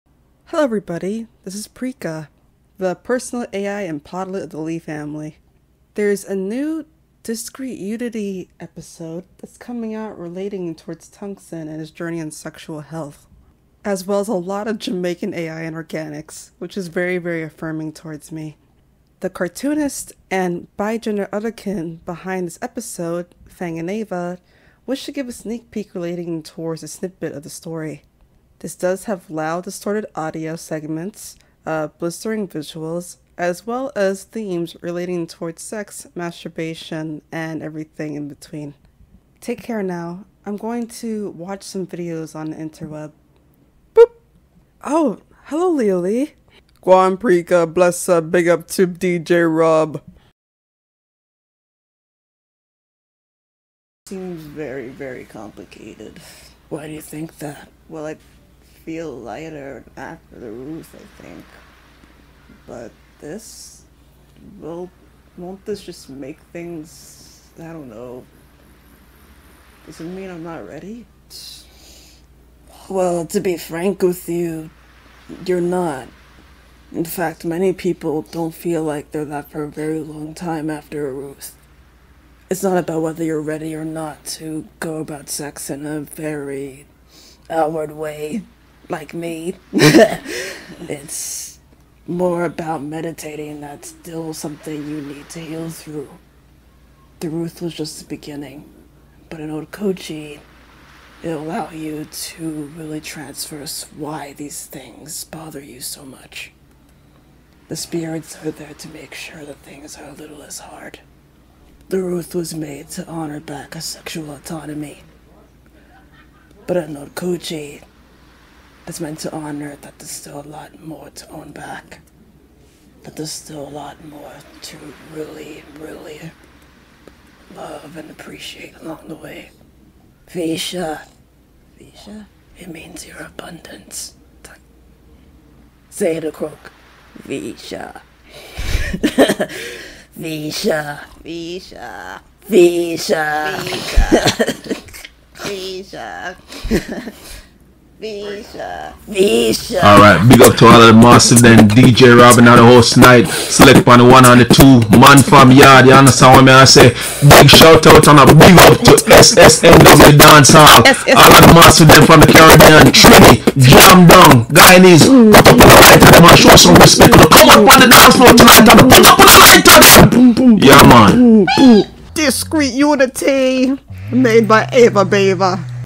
Don't Go_Discrete Unity {Sneak Peek} Animatic Ep
TW} Adolescent S*xual Wellbeing, Strobe, Loud/Distorted Audio

Tungsten has a deep talk with his Birth Parent and Zebbe, Dipstik, about heading into the S*xual world with Unraveled anxiety! After the Ruthh that TJ has experienced, he decided that having an Ode Chip inside him will help his healing journey even more! Inside he already understands that his pleasures are okay, but seeing where it will go is the lifelong choice that he knows he must take

Animatic} by Ava Beyva
Editing and SFX} by Ava Beyva
Voice Acting}
_Tungsten and Dipstk} Ava Beyva
_DJ ROB} by Ava Beyva's Jamaican Father! Wade :D